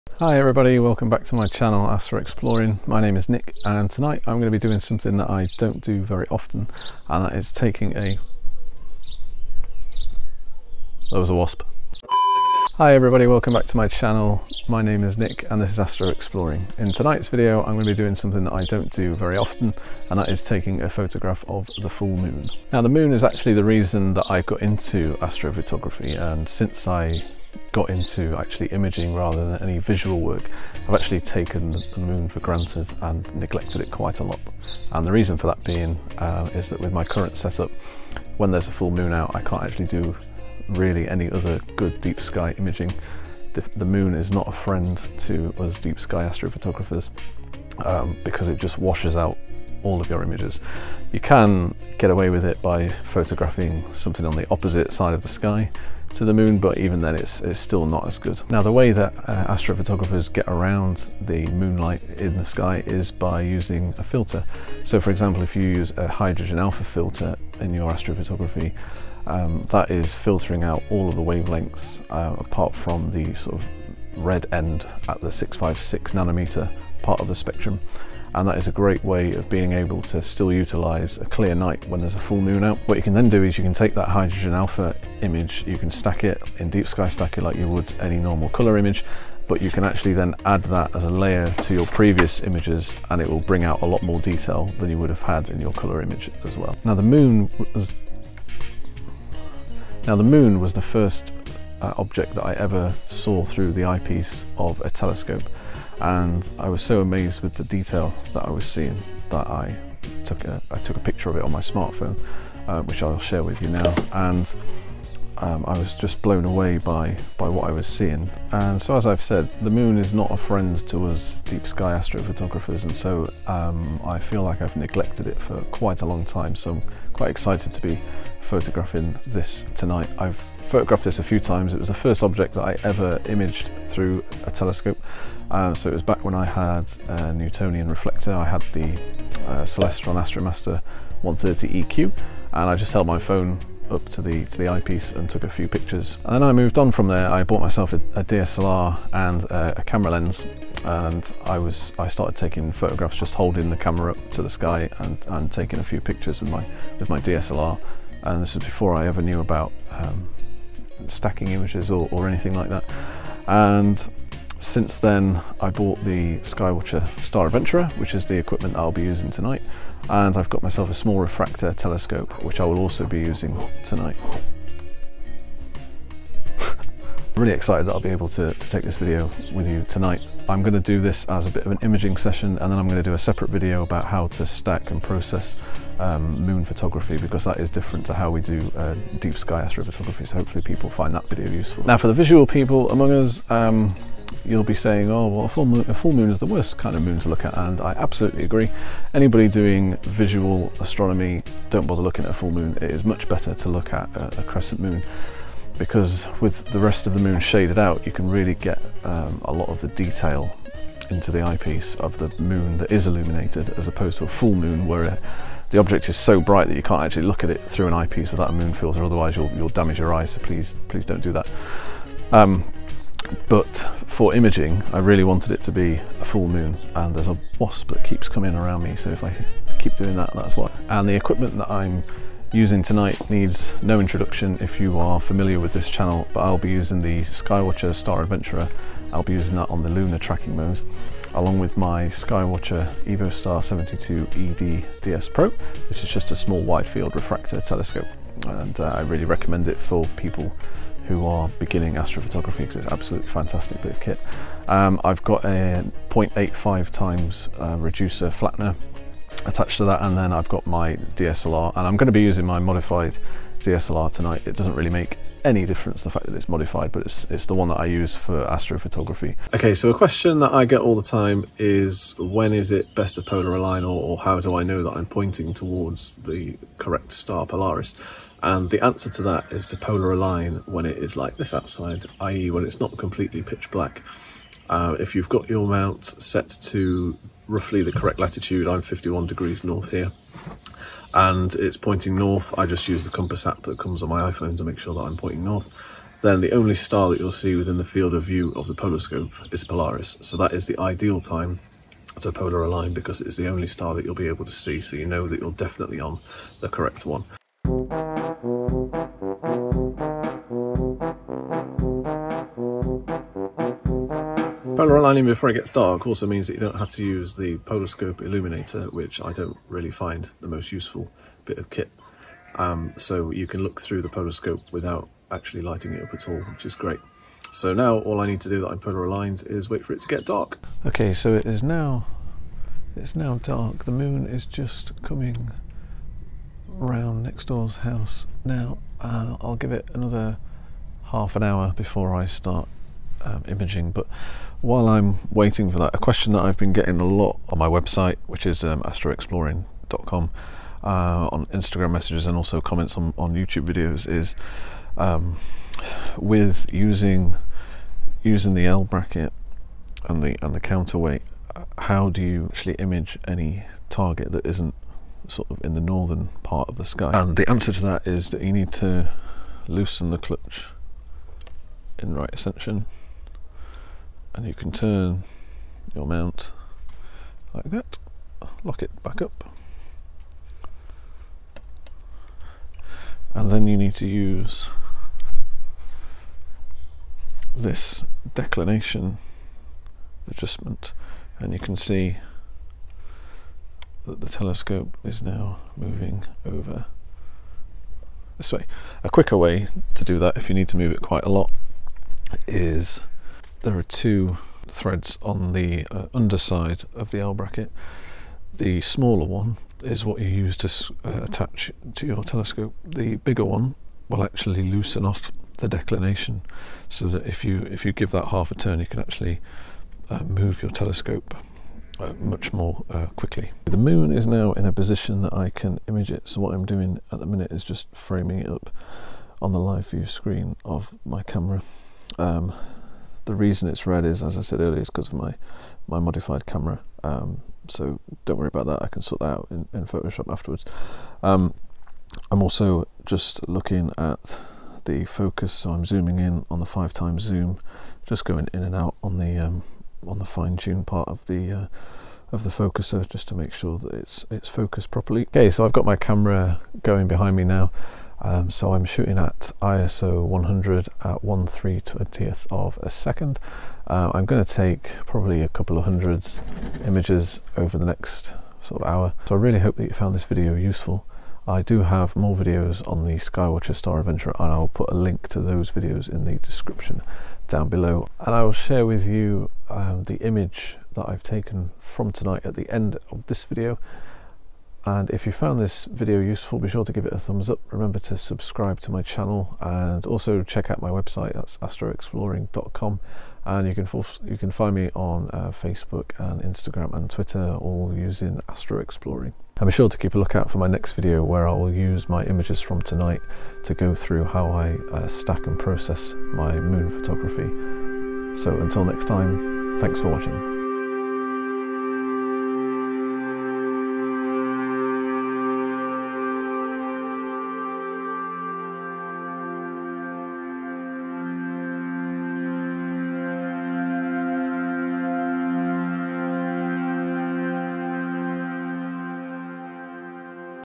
Let's Photograph the Moon with a Telescope
Taking a photograph of the full moon with a telescope is a hugely rewarding experience. However, you don't need fancy equipment to photograph the moon, you can just use your smart phone! This is what makes the moon a perfect astrophotography target, it's accessible to anyone!

In this video I give an overview of my process of imaging the moon using my Sky-Watcher Evostar 72ED DS-Pro and Sky-Watcher Star Adventurer.

US based viewers.